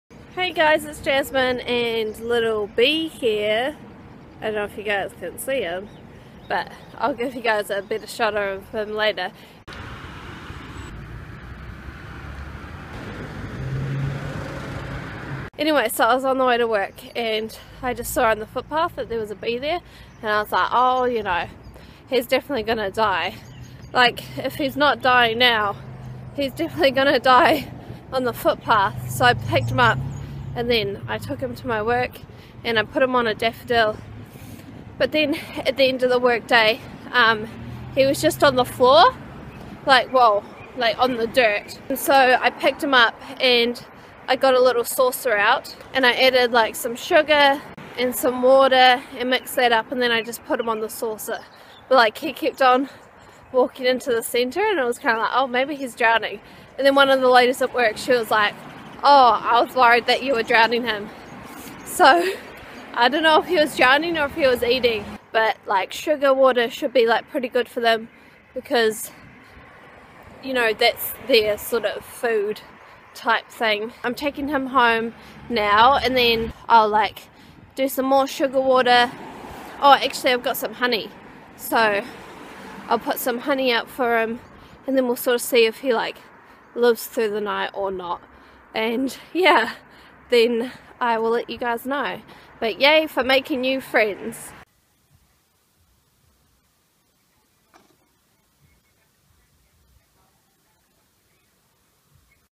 Saving A Bee And Making Friends In New Zealand | How Do You Do That?!
Hey, everybody!

Today I found a bee on my way to work. He was looking pretty tired so I found him a nice daffodil for him to eat from. At the end of my work day, I found him lying on the dirt again. I decided to place him on a dish of sugar water so he might get some energy back. I'm on my way to take him home now so that he can recuperate and be fighting fit. I'm not sure if he will last very long though as he seems very tired and unwell. All I can do is keep him fed and warm and hope he flies away or dies peacefully.

I hope all the bees out in the world are doing okay and they have enough flowers and pollen and are free of parasites. One can only hope.